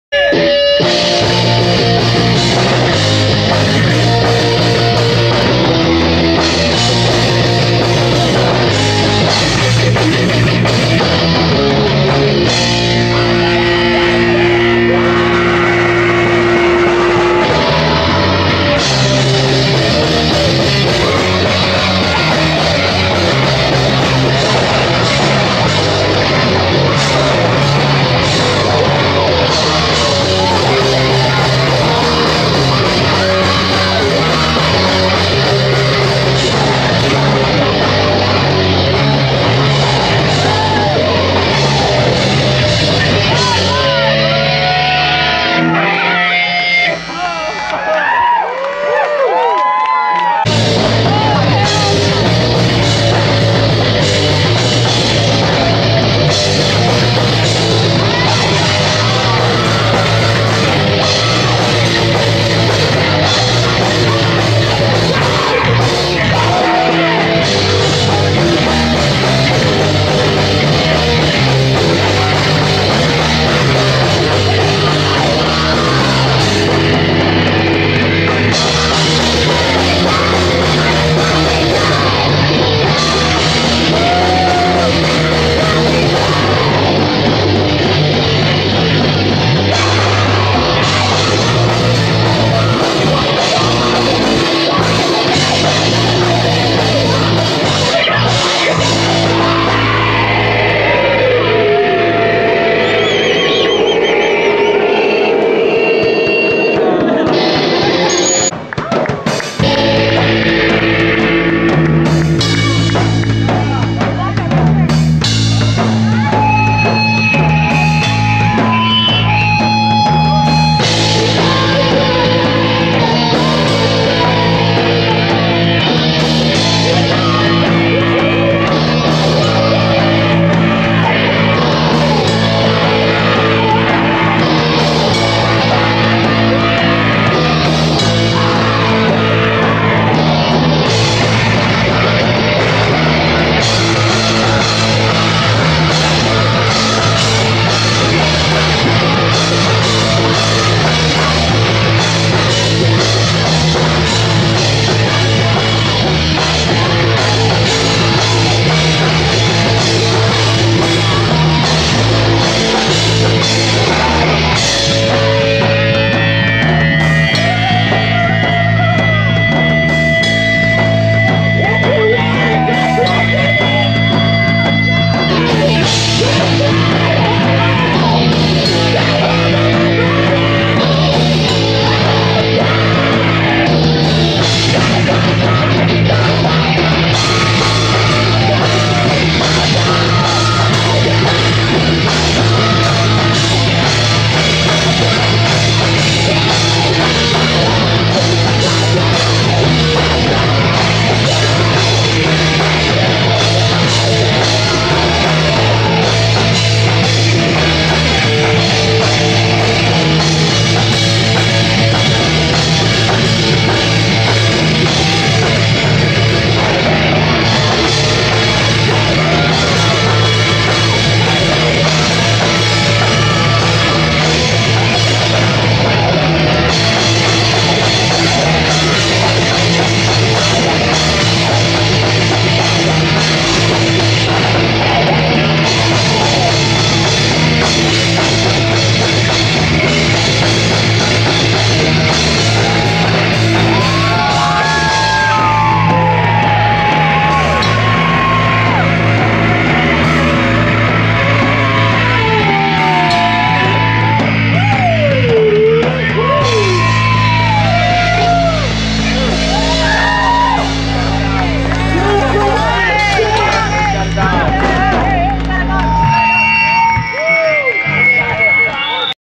george crustanza @ clarion alley block party (SF, CA) 9-21-19
Here are some songs by Andy Human and the Reptoids playing at the 2019 Clarion Alley Block Party in San Francisco